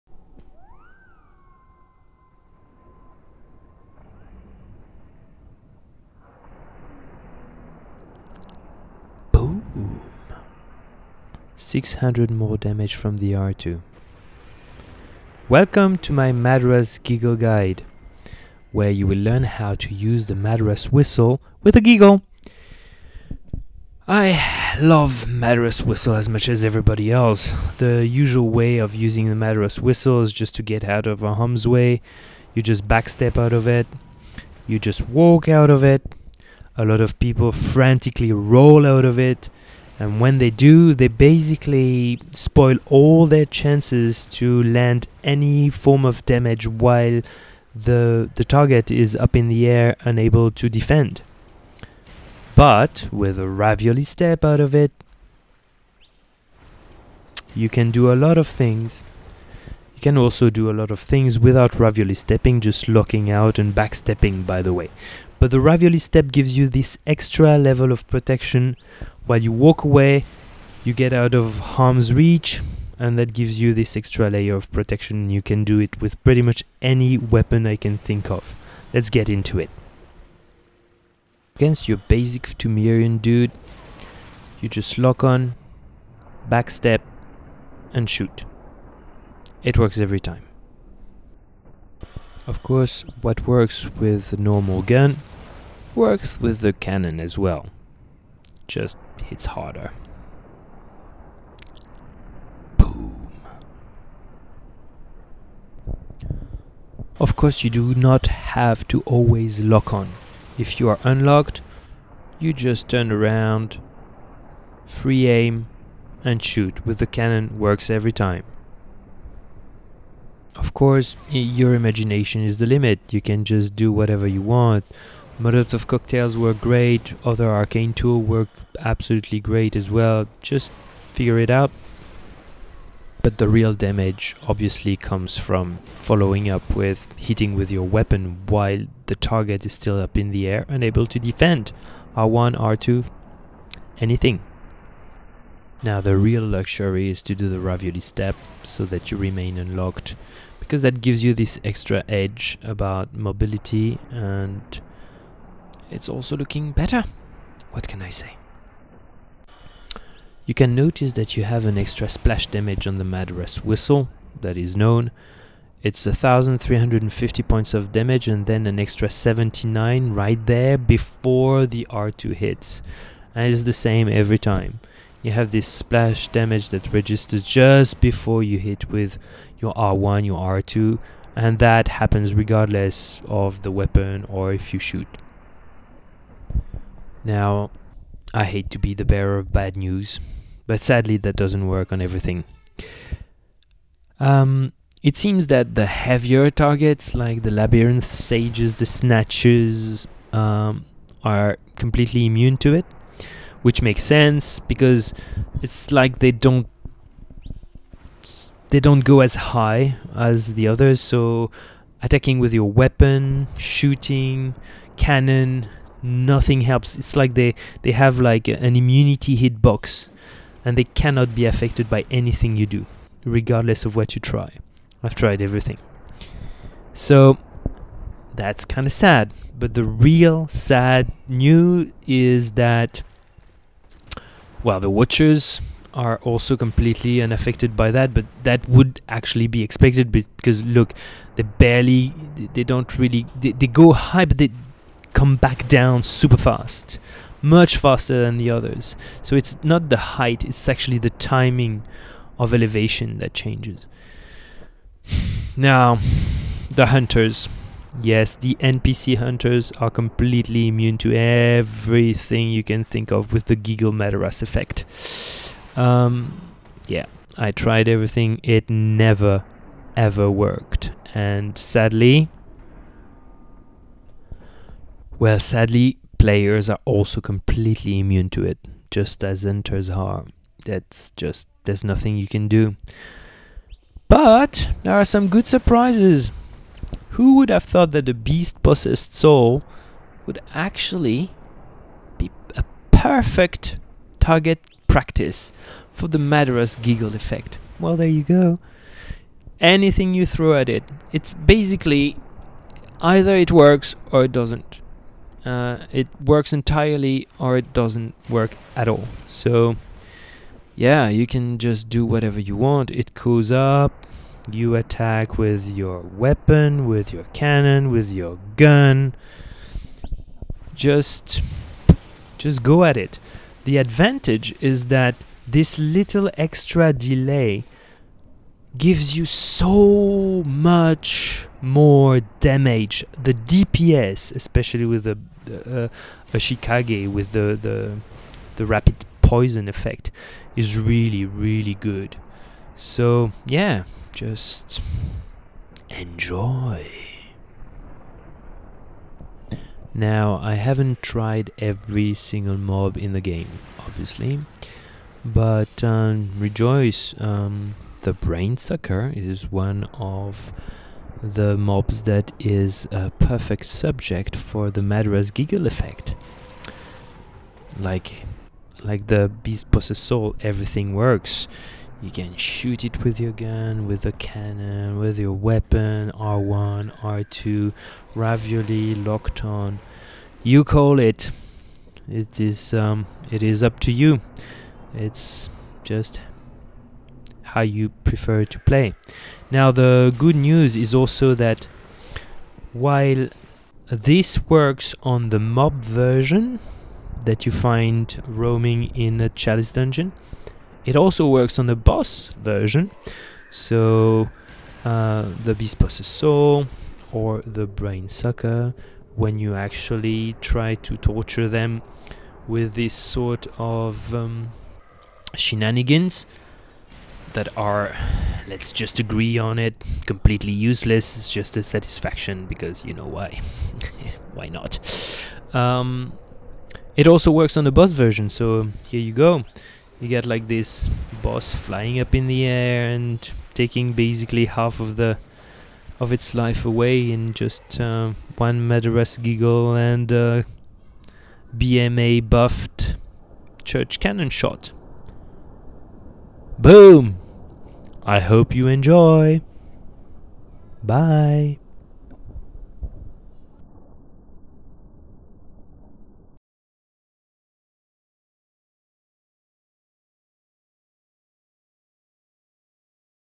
MADARAS WHISTLE-advanced tool guide-useless but fun
this is all about fun, using the madaras whistle hitbox frames to trigger true combos on selected mobs. Sadly hunters, npc or actual players are immune to those combis due to hitbox ligger being way shorter for them (sadly) but some mobs like beast possessed soul, brain sucker etc are susceptible to it, opening for massive damage burst to dispatch those morons with flair in one go.
You would think that the weight would be the only factor to be able to trigger true combos but nope: beast possessed soul is open to it while a small hunter is not...go figure. another discreapancy to avoid PvP abuse (by that I mean reward for good players sadly) but anyways.
I hope you will learn some things there and that you will want to experiment this on your side. the idea is to encourage ambitious playing rather than the usual dull safe moves and sequences you always see.

a hunter must hunt...just do it with flair god dammit!

Timecodes
0:00 - introduction
1:19 - basic locked on shot
1:46 - unlocked canon shot
2:00 - arcane tools combo
2:13 - locked backstep R1-R2
2:25 - unlocked ravioli step giggle
3:08 - resisting mobs (snatchers, sages, watchers, hunters)
4:42 - works on BPS
5:50 - works on Brainsucker (mob)
7:06 - works on brainsucker (boss)